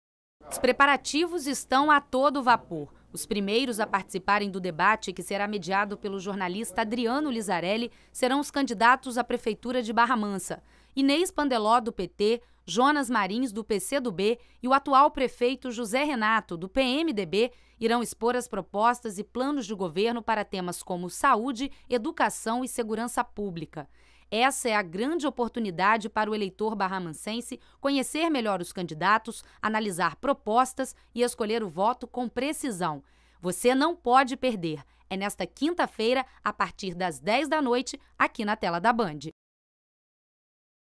Preparativos finais para o debate com candidatos de Barra Mansa
Já está quase tudo pronto para o primeiro debate das Eleições 2012 do interior do estado. Confira!

Perdeu o debate? Clique no seguinte link e confira na íntegra